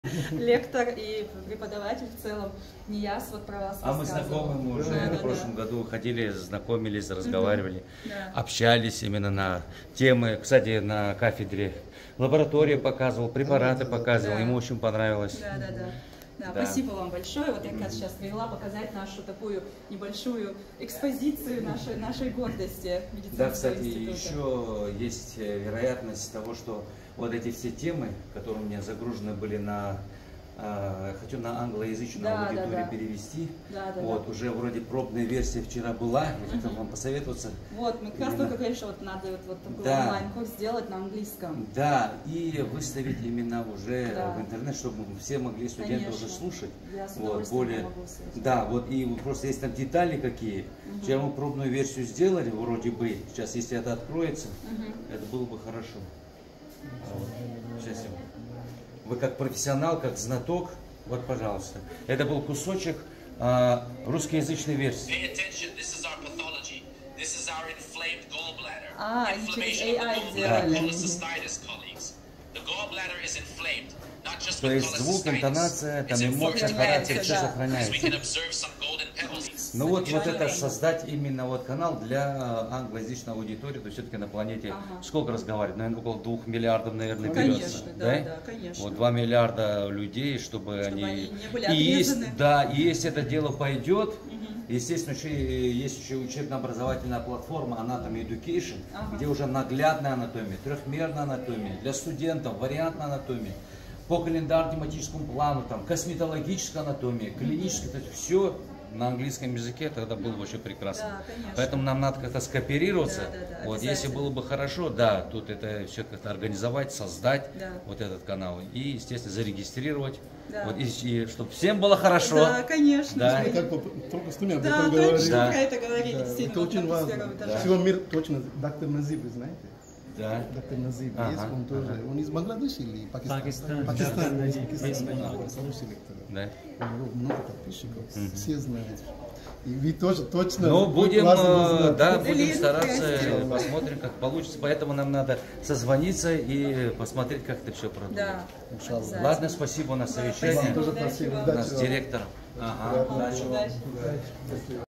Anatomy Professor Edgar Kafarov Chechen State University
Doctor of Medical Sciences, Professor, Head of the Department of Normal and Topographic Anatomy with Operative Surgery, Deputy Director of the Medical Institute at the Chechen State University for Scientific and Organizational Work;

Member of the All-Russian Scientific Society of Anatomists, Embryologists, and Histologists, Chairman of the regional branch of the Chechen Scientific Society of Anatomists, Embryologists, and Histologists; Member of the North Caucasus Scientific and Educational Medical Cluster (Stavropol).

Since 2018, Deputy Editor-in-Chief of the scientific and practical journal "Bulletin of the Medical Institute."

Since 2018, a member of the editorial board of the American journal "American Journal of Bioscience and Bioengineering."

Winner of the All-Russian competition "LEAGUE OF LECTURERS - 2022" for the title of "Best Lecturer."